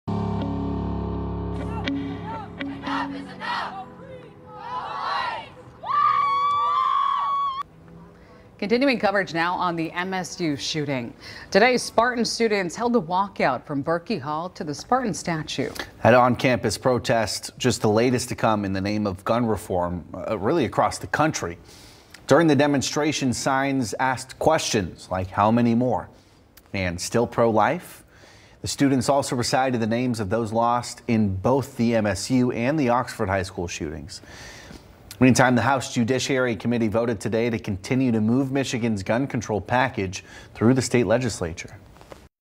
MSU students hold walkout, demand action after shooting
Michigan State University students held a walkout Wednesday afternoon demanding action in an effort to end gun violence.